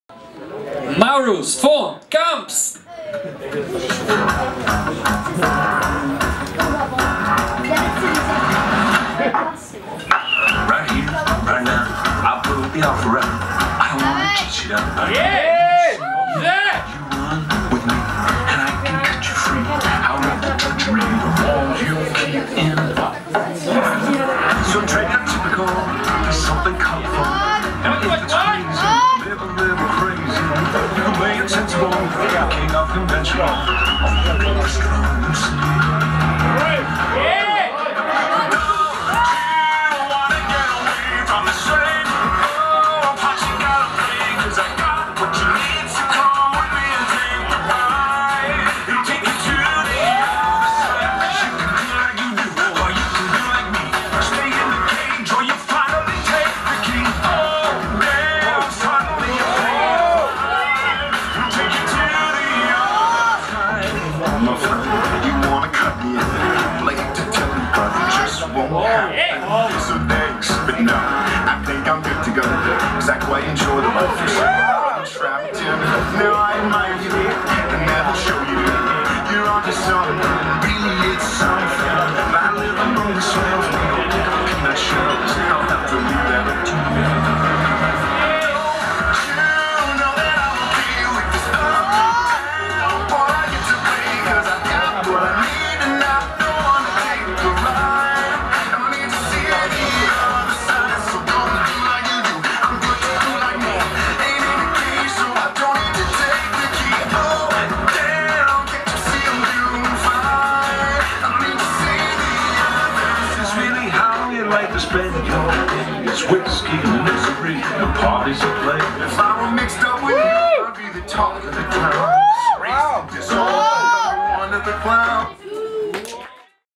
CYC BRNO 2022 - 1A PRO - 19th Maurus von Kamptz
Czech Yoyo Cup 2022 Brno
Freestyle ze soutěže CYC, který se konal 9.4. v Brněnském klubu Music Lab.